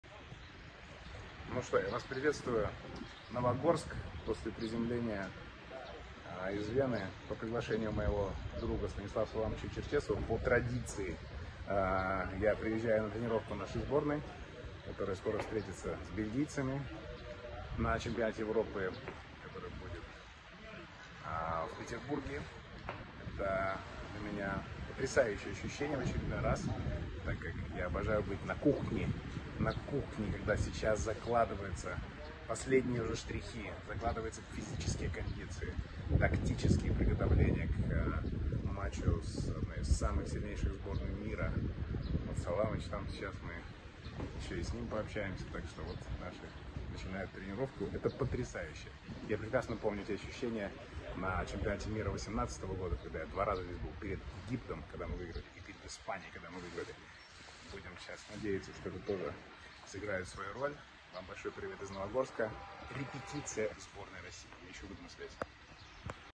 Видеоблог Дениса Мацуева из Новогорска
Денис Мацуев на тренировке сборной России по футболу.